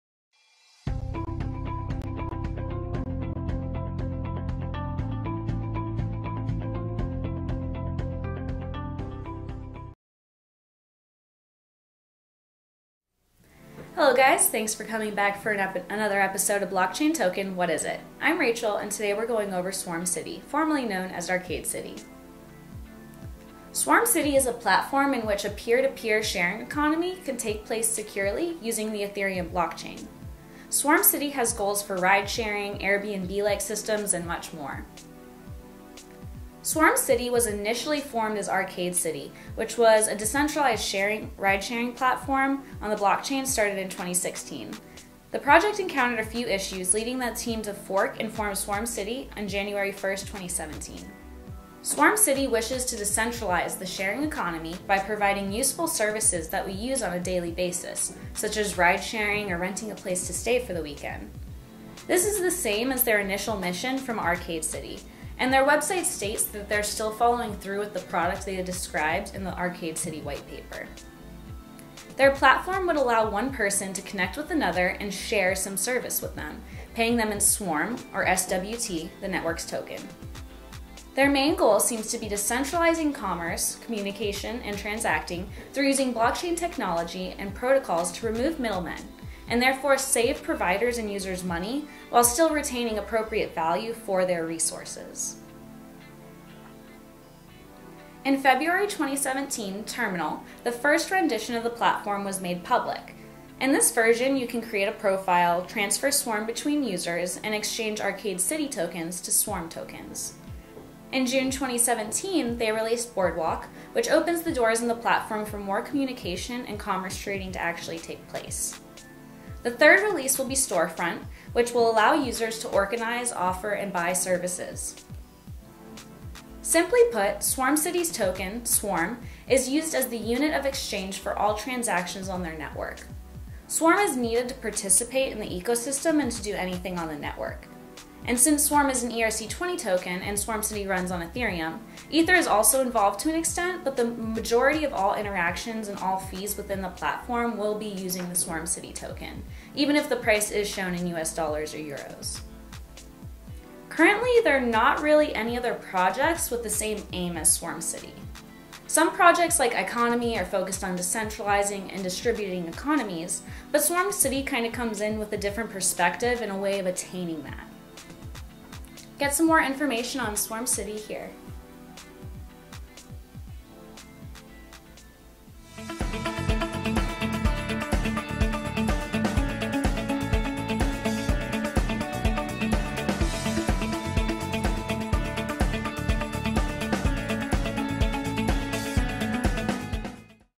Swarm City - Blockchain Token: What is it?
Originally formed as Arcade City, Swarm City is a platform in which a peer-to-peer, sharing economy can take place securely using the Ethereum blockchain. Swarm City has goals for ridesharing, AirBnB like systems, and more.

If you enjoyed this video, please give us a “Like.” Check out some of our other Blockchain Token episodes, and learn more about different blockchain projects!